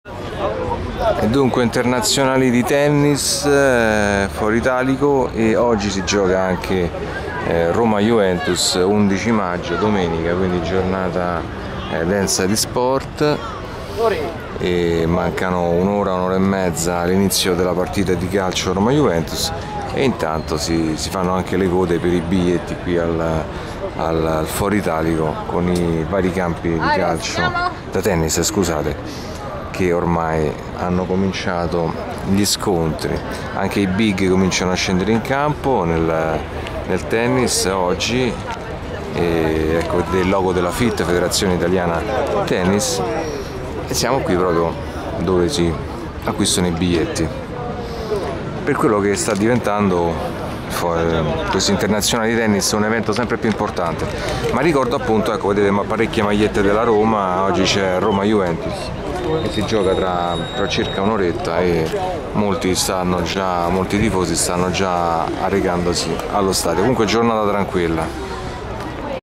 Internazionali Tennis Foro Italico e Roma Juve Stadio Olimpico
Roma Juve all'Olimpico e in contemporanea gli internazionali di Tennis al Foro Italico, video delle code alla biglietteria.